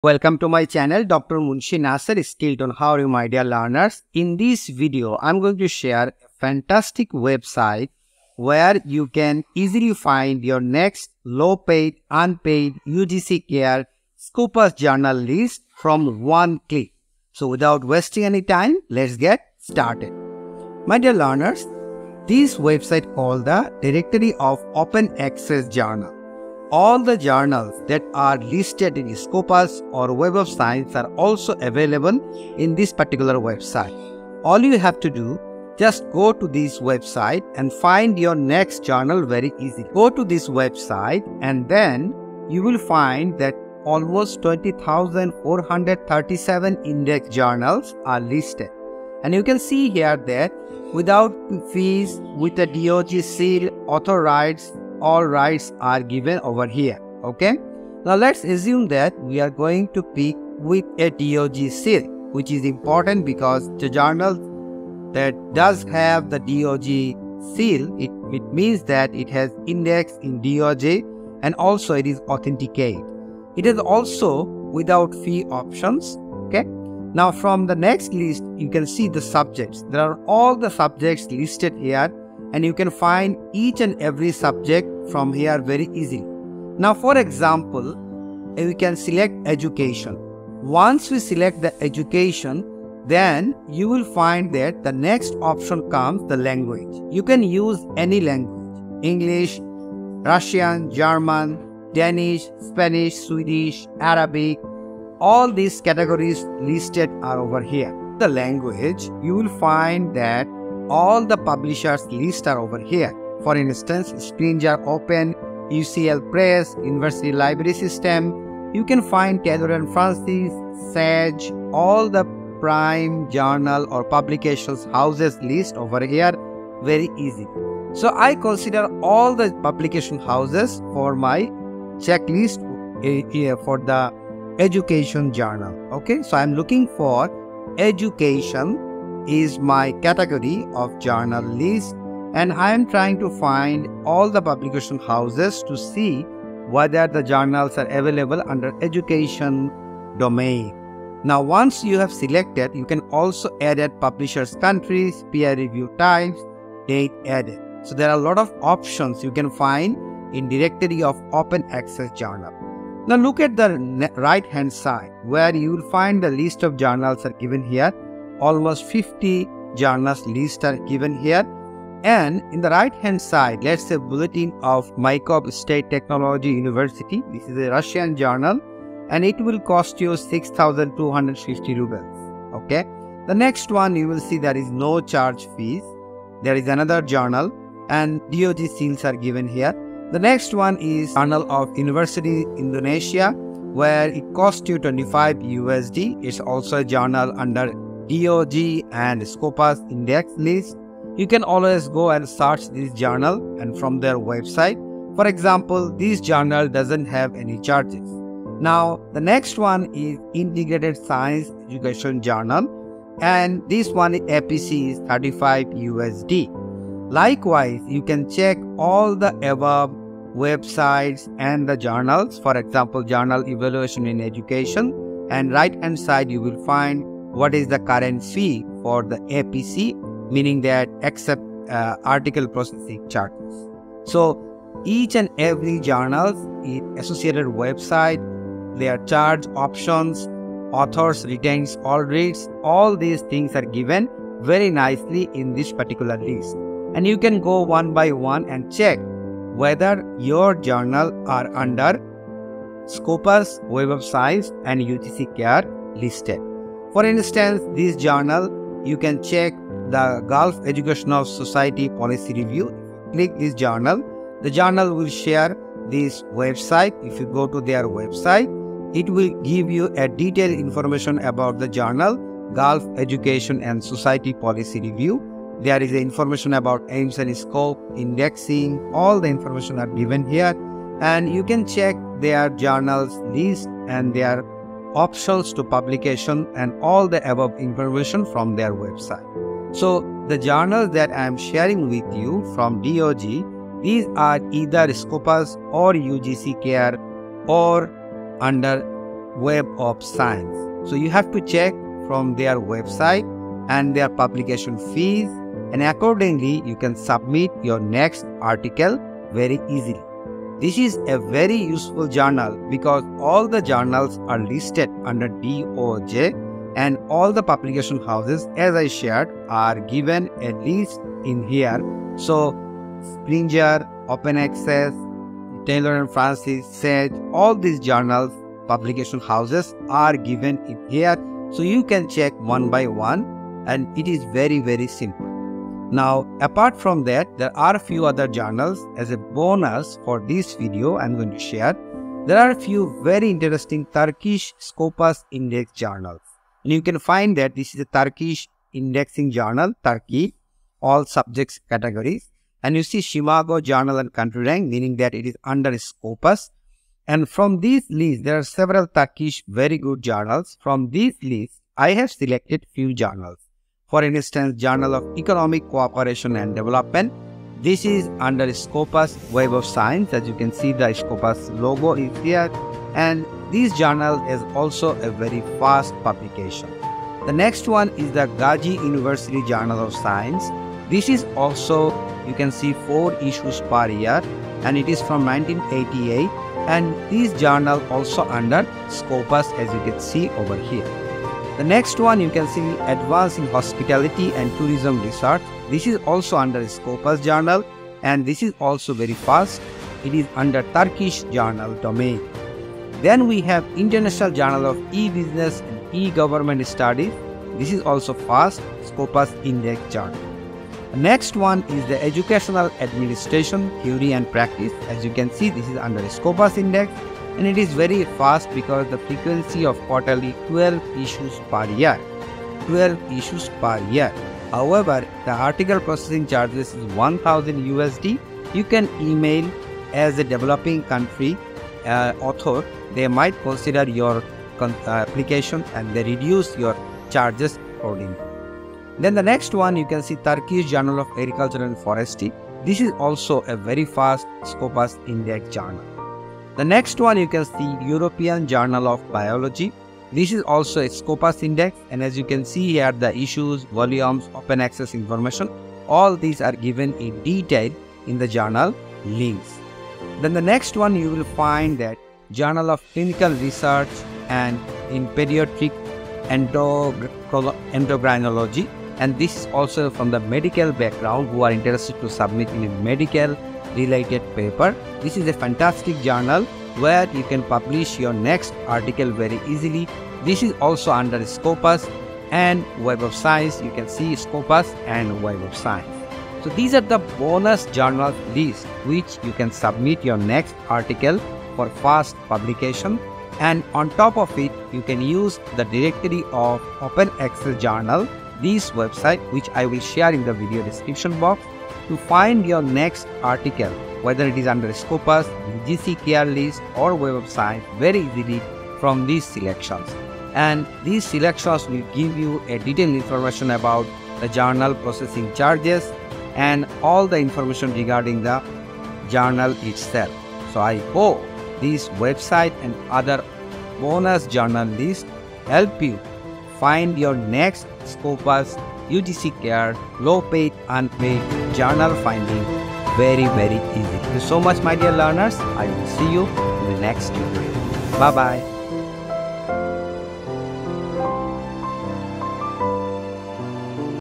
Find Your Low-Paid/Free Scopus/UGC/WOS Journals Very Easily| Check the Fast Journal list from Turkey
In this video, I have shared the DOAJ website, where you can find your next low-paid or free Scopus/UGC Care listed journals very easily. DOAJ is a unique and extensive index of diverse open access journals from around the world, driven by a growing community, and is committed to ensuring quality content is freely available online for everyone. DOAJ is committed to keeping its services free of charge, including being indexed, and its data freely available.
I have added fast Scopus Turkish journals for your next article submission as a bonus in this video.